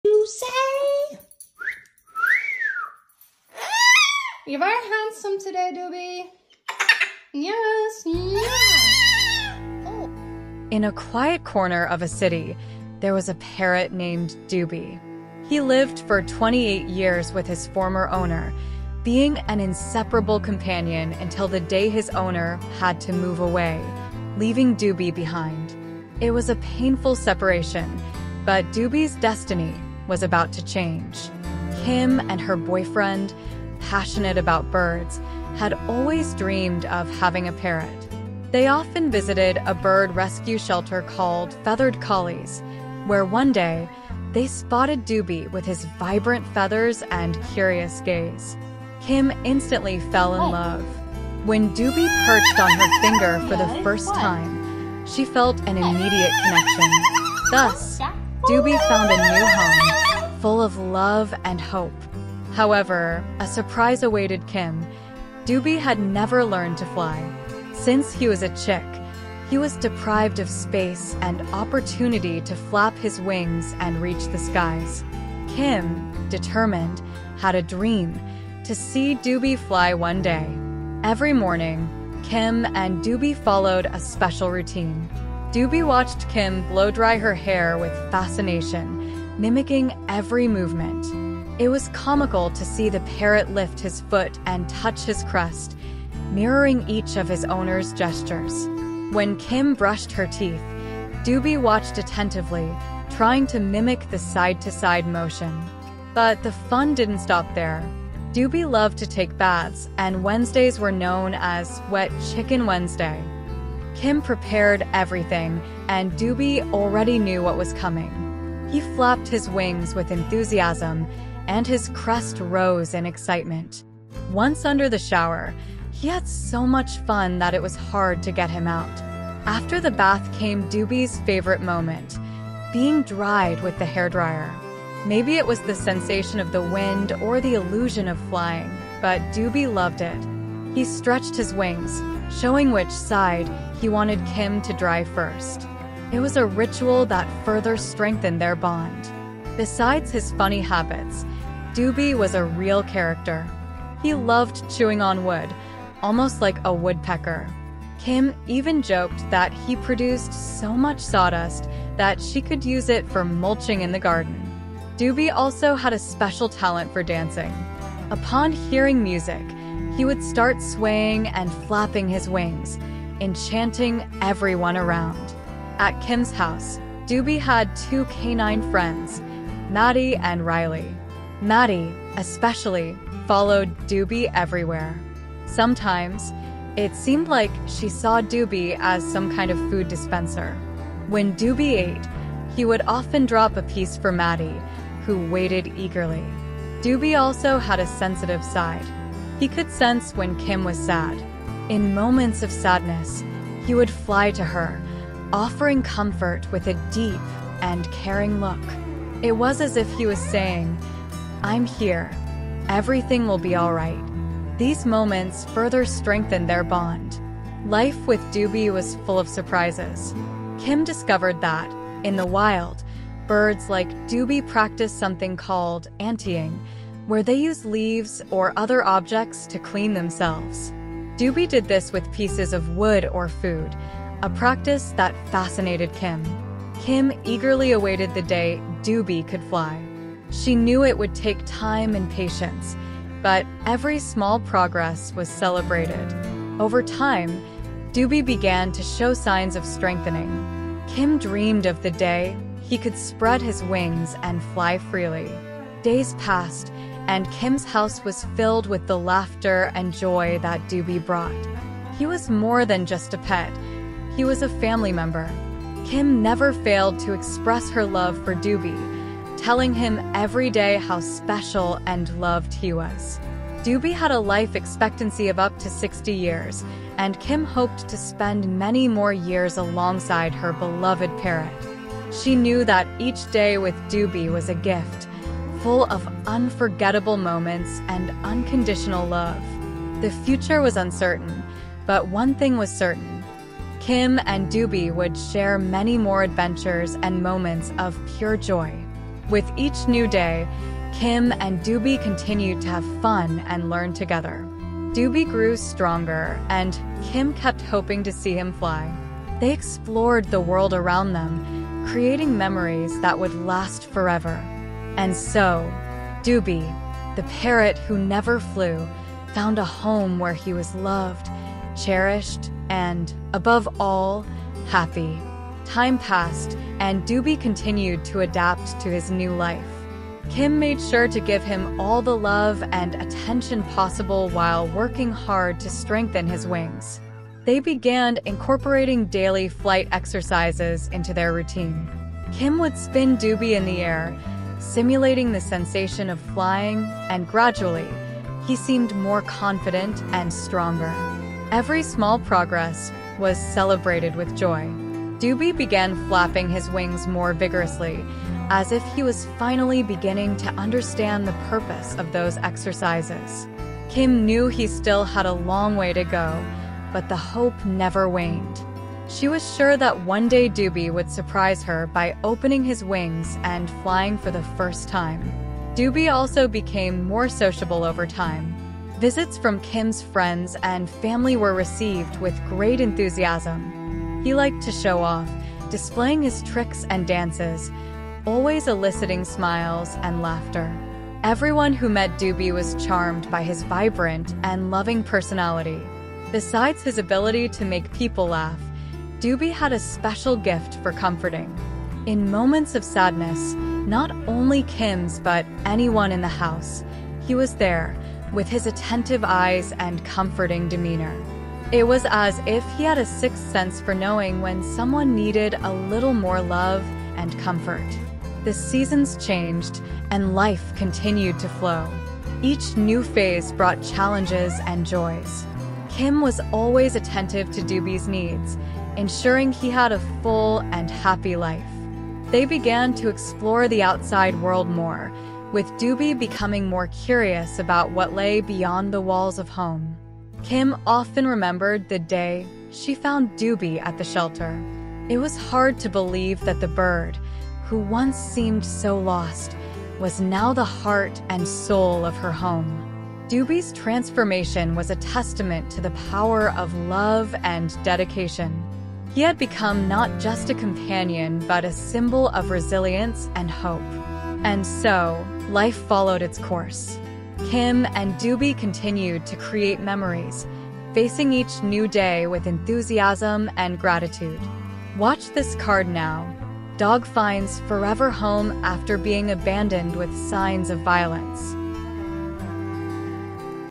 The elderly abandoned cockatoo doesn't know how to fly. You won't believe what happened next.
In a quiet corner of a city, there was a parrot named Doobie. He lived for 28 years with his former owner, being an inseparable companion until the day his owner had to move away, leaving Doobie behind. It was a painful separation, but Doobie's destiny was about to change.Kim and her boyfriend, passionate about birds, had always dreamed of having a parrot. They often visited a bird rescue shelter called "Feathered Collies," where one day, they spotted Doobie with his vibrant feathers and curious gaze. Kim instantly fell in love. When Doobie perched on her finger for the first time, she felt an immediate connection. Thus, Doobie found a new home, full of love and hope.